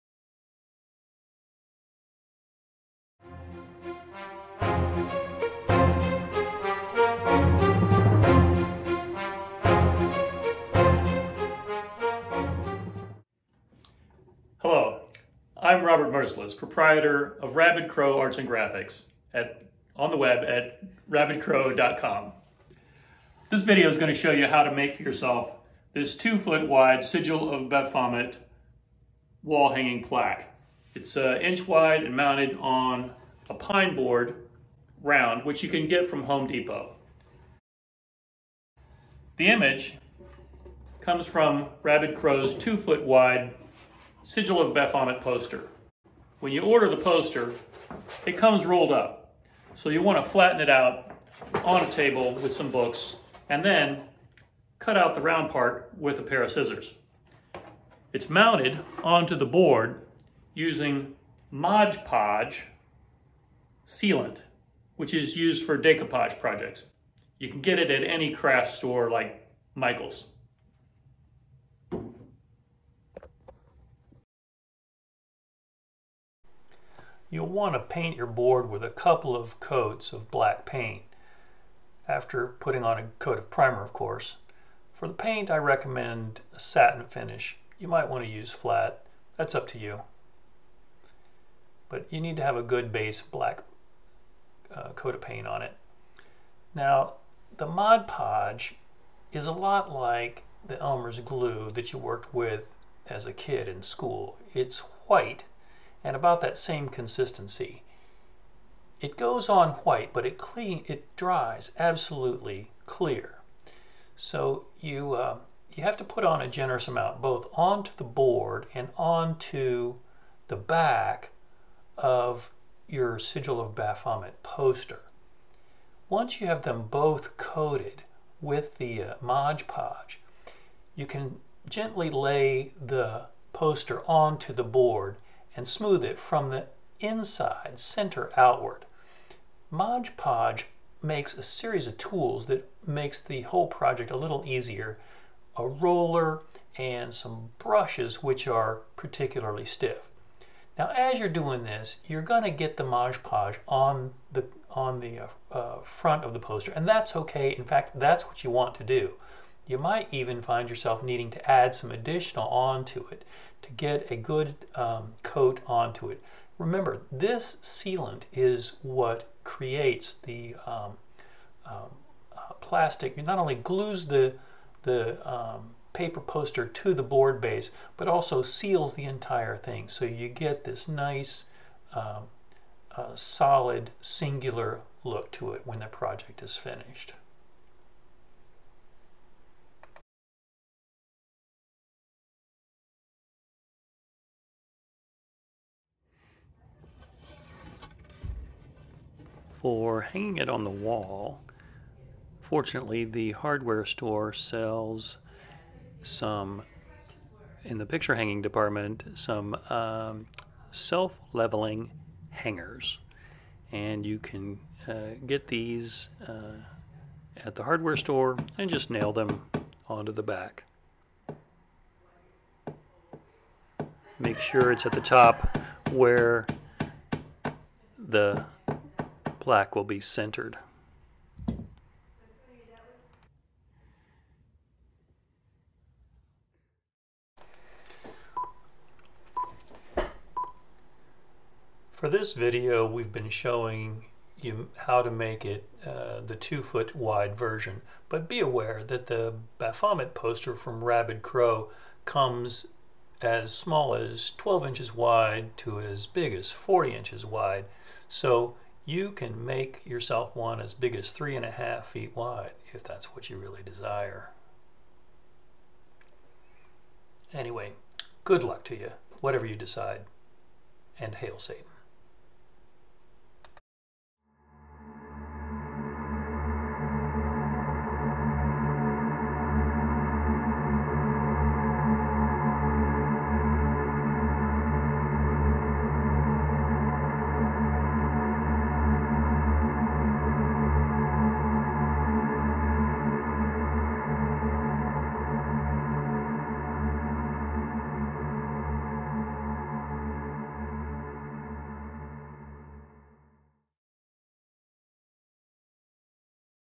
Make a Baphomet Altarpiece Wall Plaque for Satanic Ritual with Rabid Crow's "Sigil of Baphomet" art
Shows you how to make a high-quality, 2-foot wooden "Sigil of Baphomet" wall plaque / altar piece for home decoration or for use in actual Satanic rituals. Starting with a black painted, round, 24-inch wide wooden pine board, you simply use Mod Podge decoupage sealer to mount the 24-inch sigil poster from Rabid Crow. The poster is the official "Sigil of Baphomet" licensed by the Church of Satan.